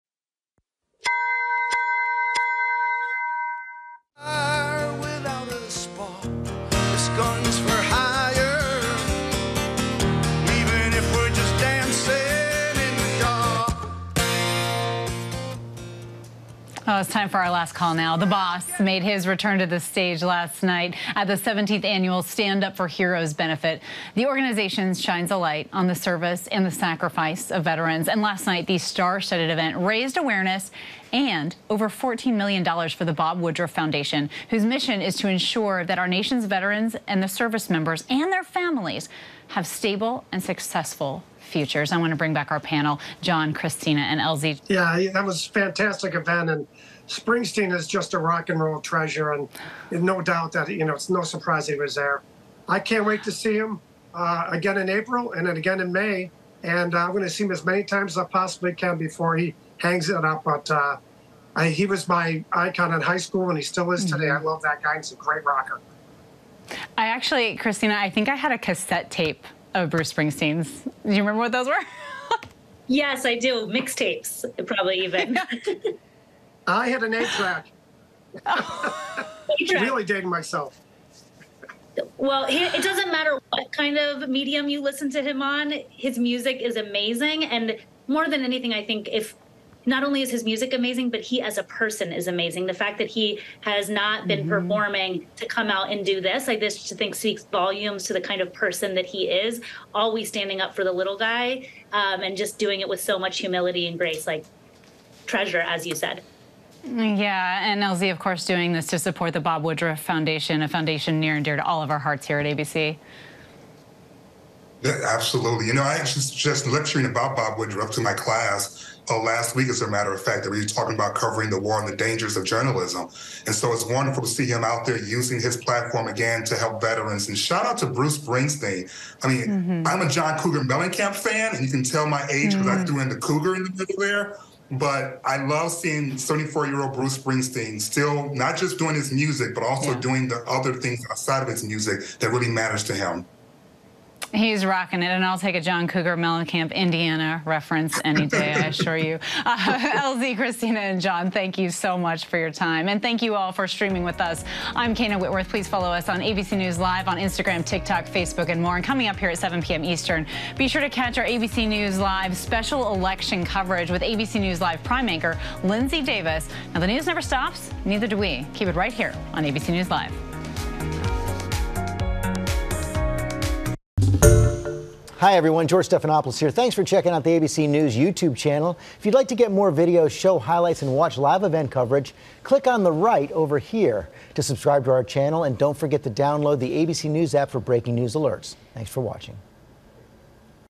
Springsteen returns to the stage for a good cause
Bruce Springsteen's battle with a peptic ulcer forced him to cancel shows in September but "The Boss" made an early return to the stage for the Stand Up for Heroes fundraiser.